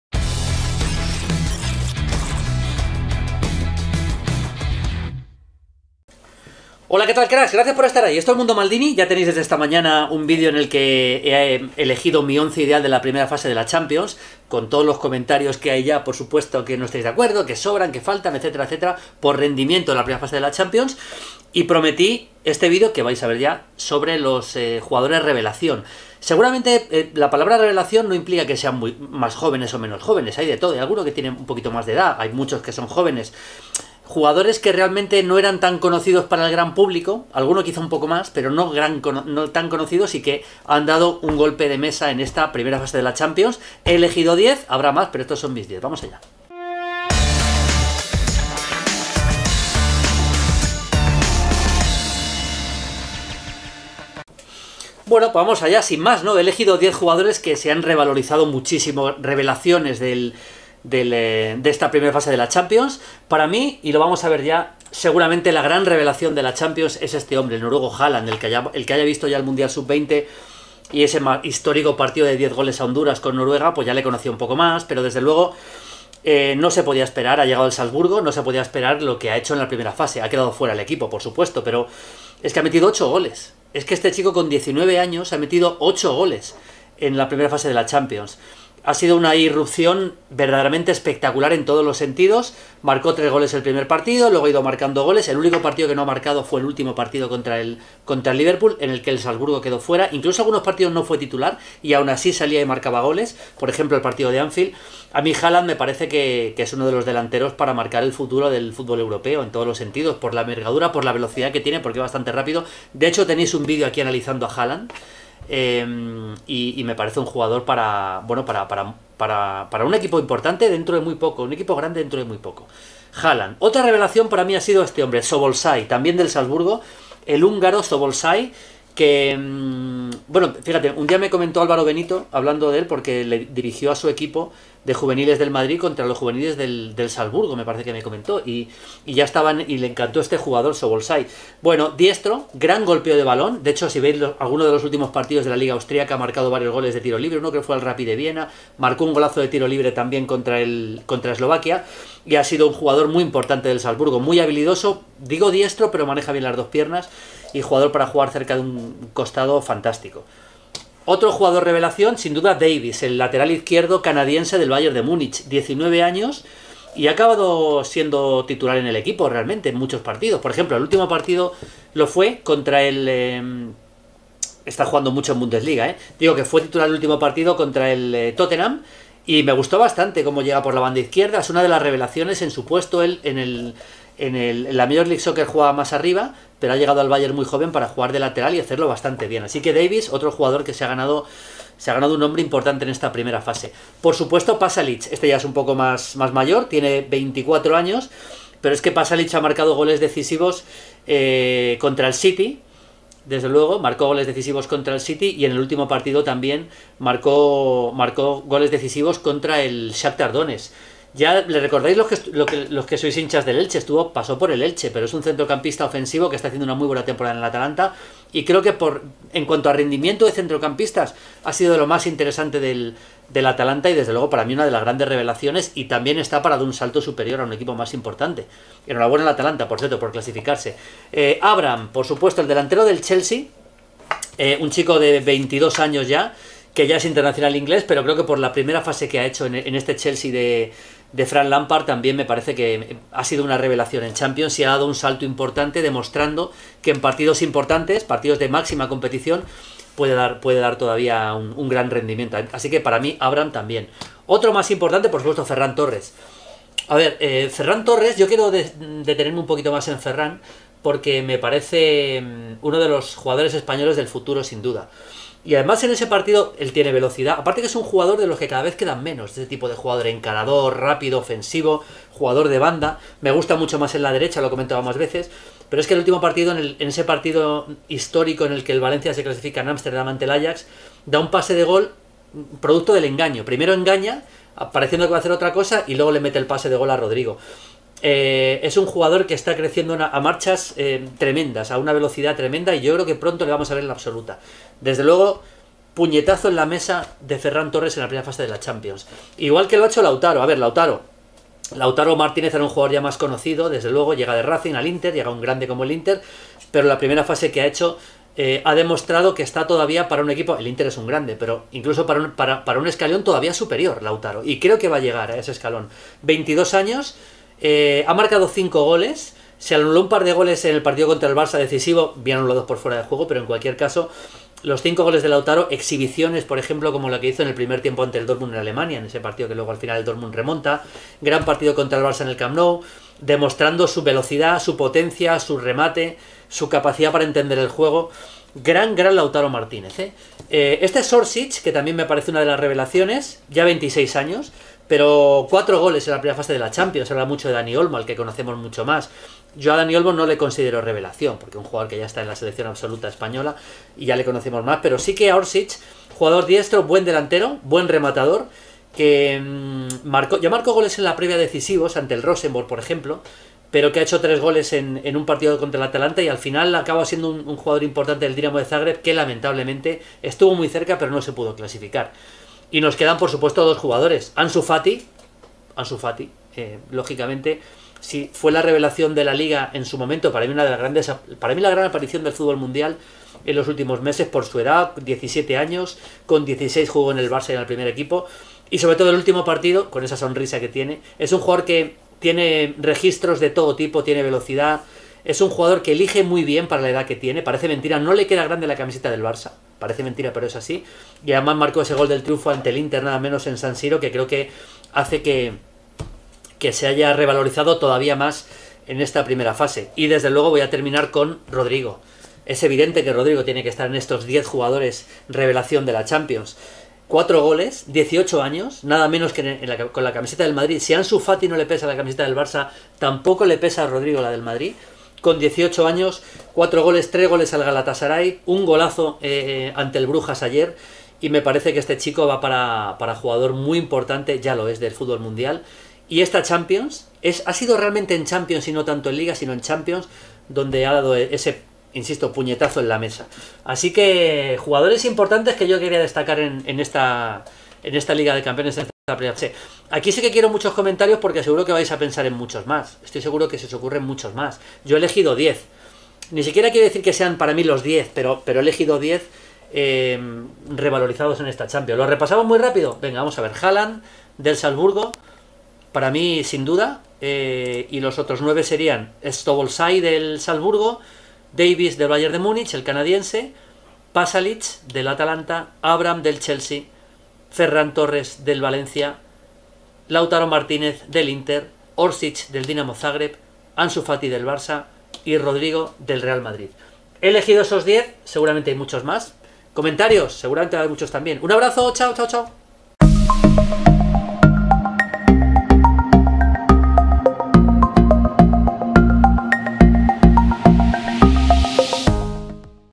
CHAMPIONS. Mis diez jugadores revelación de la primera fase. ¿A quién añadís?
Terminó la primera fase de la Champions y aquí tenéis mis diez jugadores revelación. Futbolistas que con sus actuaciones han llamado la atención muy por encima de lo esperado, no todos tienen que ser muy jóvenes. Espero vuestros comentarios porque seguro que hay muchos más que merecen entrar en esta lista. Os leo a todos cracks!